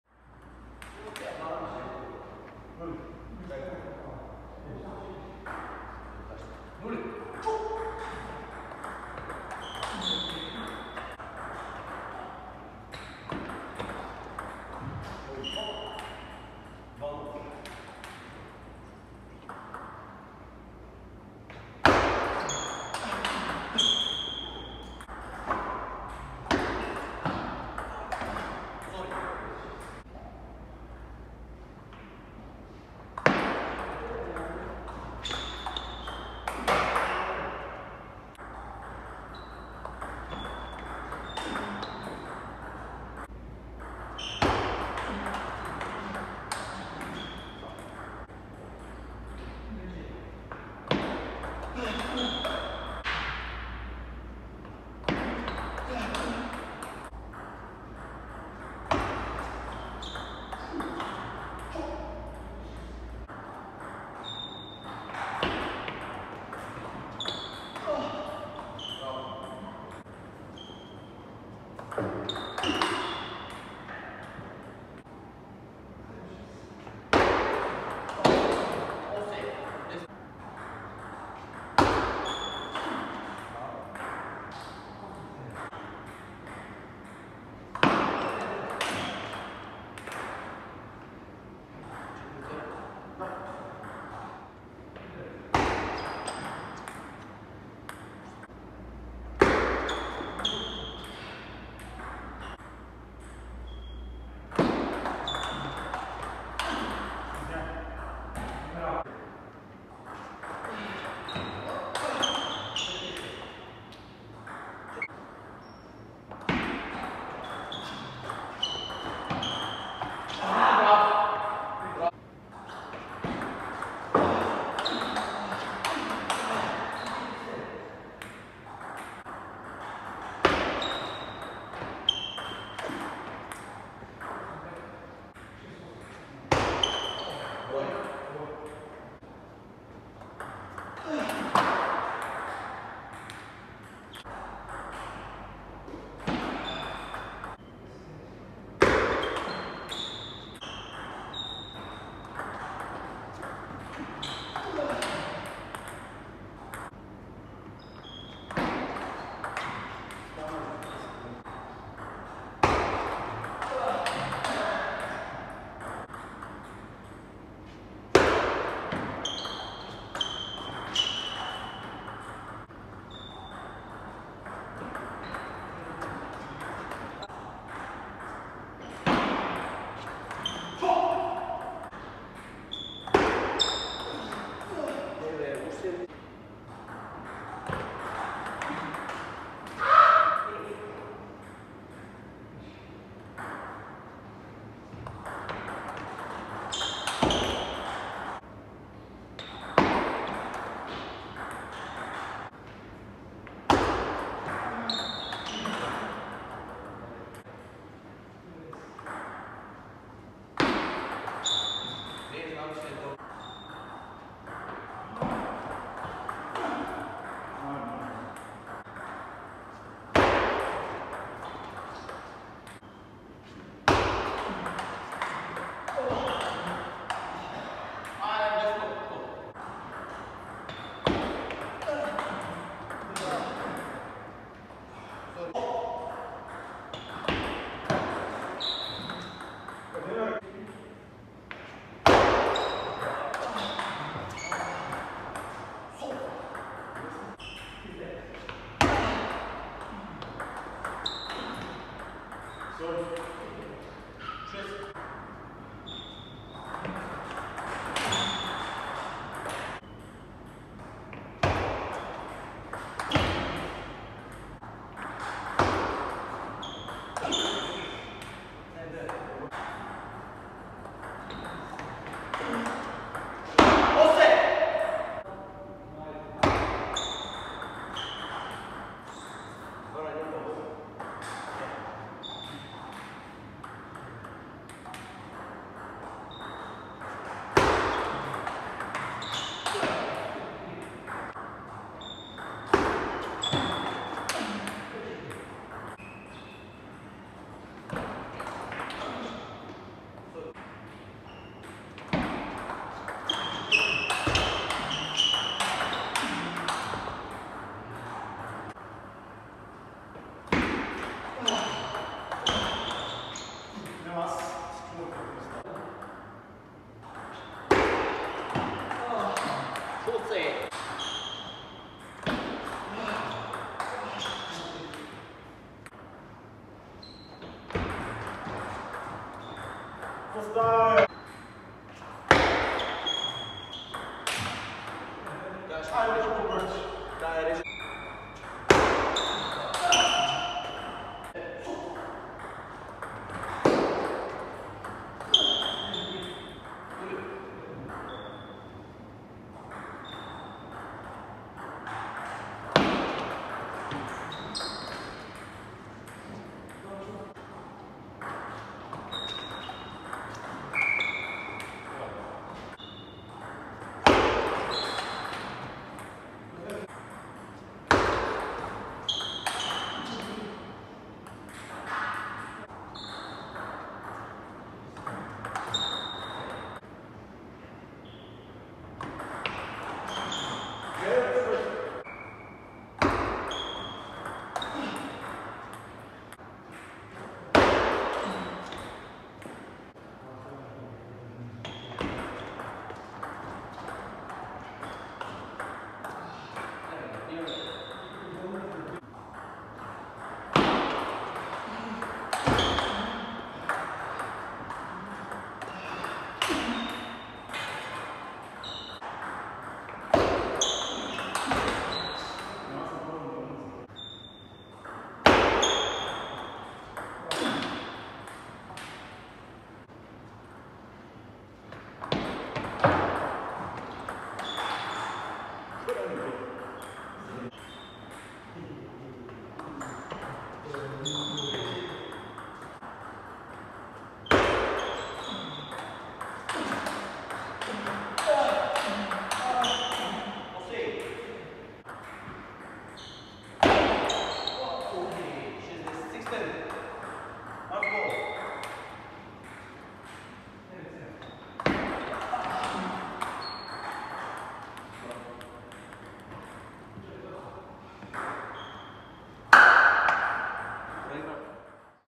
Table Tennis Practice match #1 | P. Pavlov (R) vs. P. Varbanov (L)
Results: Plamen PAVLOV  4 : 1 Plamen VARBANOV

Players:

Plamen PAVLOV - TTC Argogroup | Premier League
Plamen VARBANOV - TTC Deo | 1st. League ( A league )

Venue - TTC Argogroup Exact

Sofia, Bulgaria